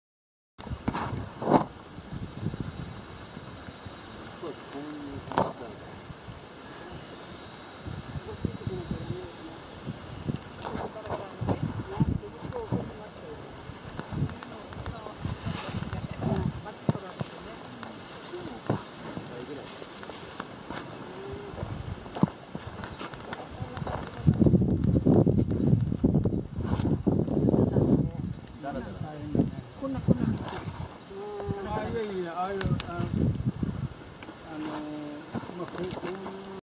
９合目手前の岩場。木曽御嶽山
９合目手前の難関である岩場です。見た目は簡単そうですが、初心者である私は怖かったですね。
転んだら大迷惑ですからね。